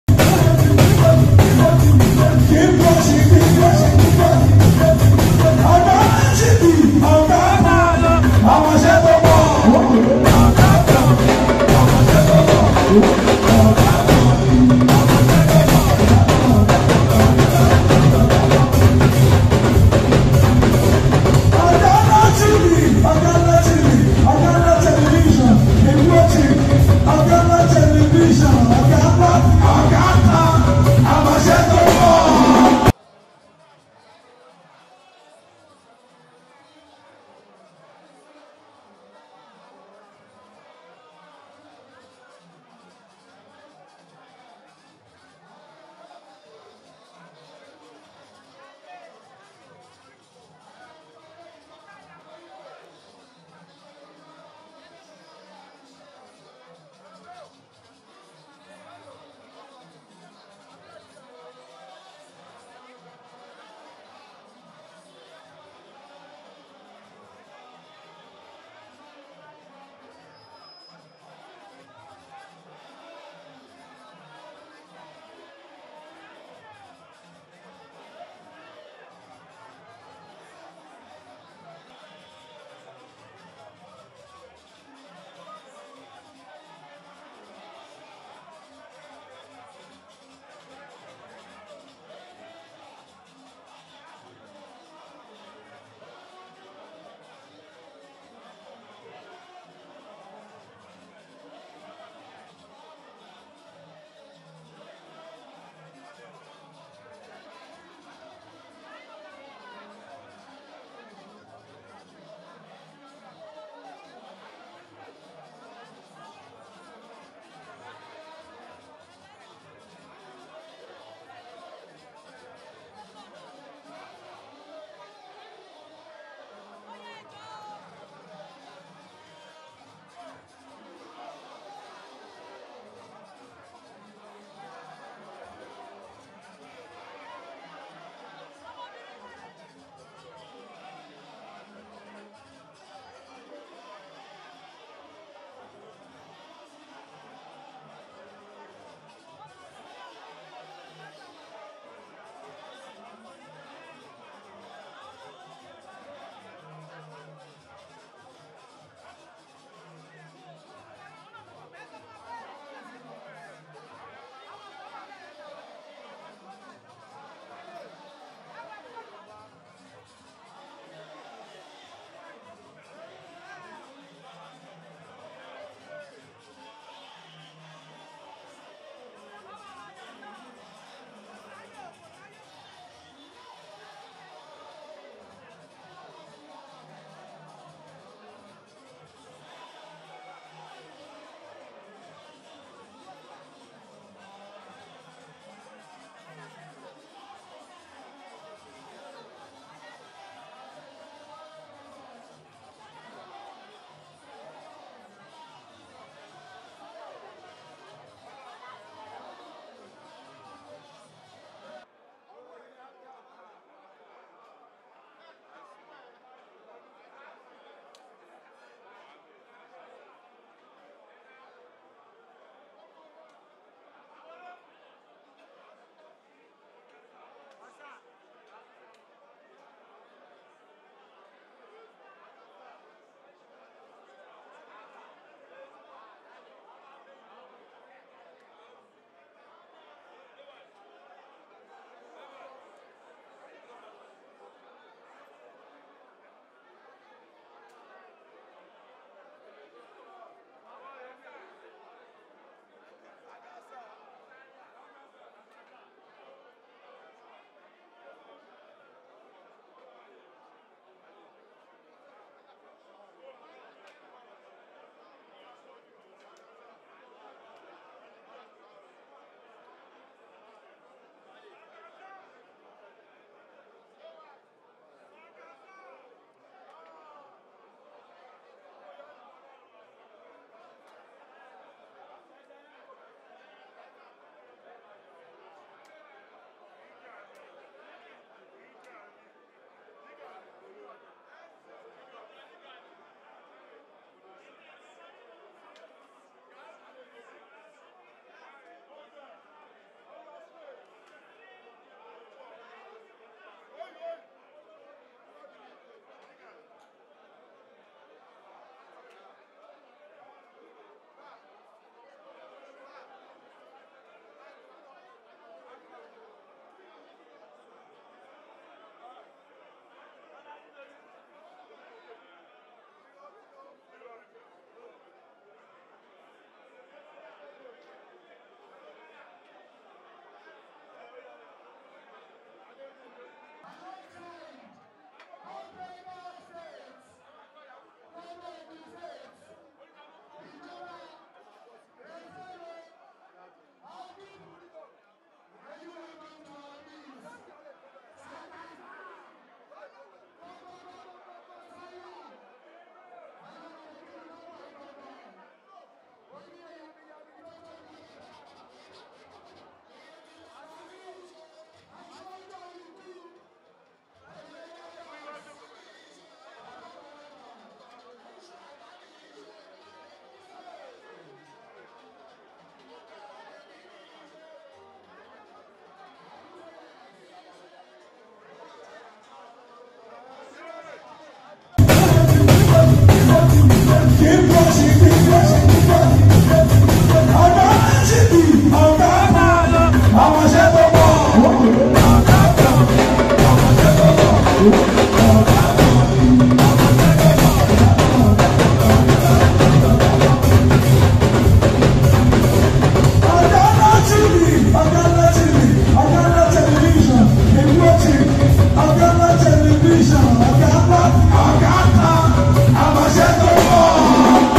STANDING OVATION FOR THE NEW N.U.R.T.W CHAIRMAN LAGOS, MUSTAPHA ADEKUNLE SEGO
STANDING OVATION FOR THE NEW N.U.R.T.W CHAIRMAN LAGOS, MUSTAPHA ADEKUNLE SEGO Oganla TV is dedicated largely to English films,original series, specials, exclusive interviews, live shows/concerts and music videos.